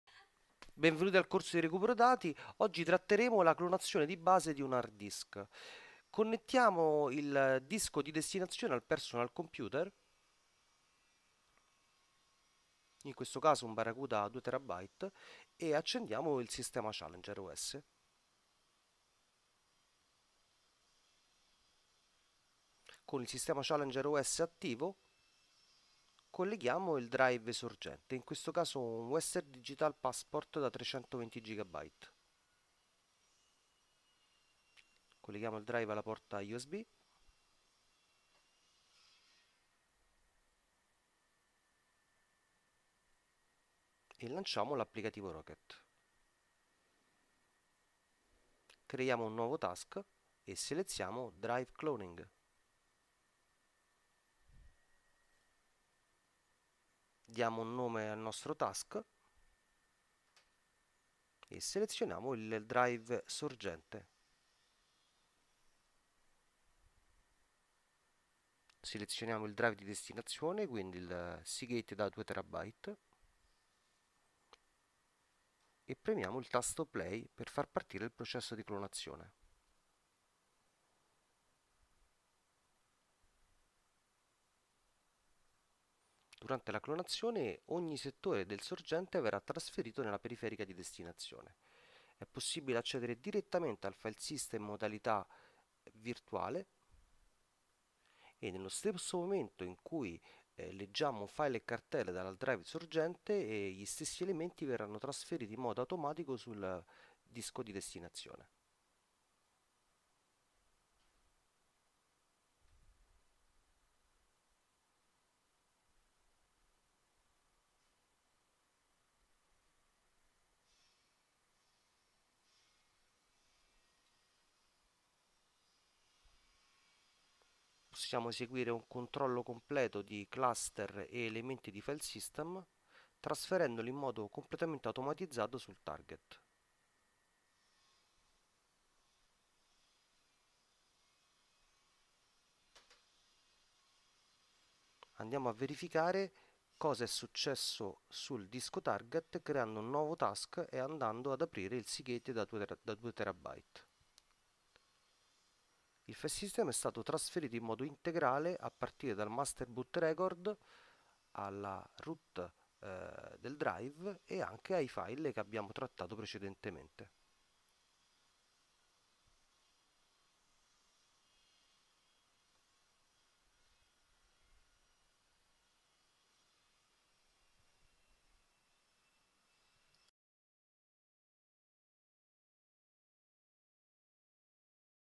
Come Clonare un hard disk con ChallengerOS e Rocket
Il processo di clonazione di un hard disk è una attività fondamentare quando si recuperano i dati o quando è necessario eseguire una analisi forense.
Challenger Rocket è il sistema numero 1 al mondo  per eseguire clonazioni professionali da hard disk danneggiati consentendo di interrompere il processo , personalizzare l'algoritmo e lo scenario di lettura e naturalmente analizzare il file system e recuperare i dati in tempo reale.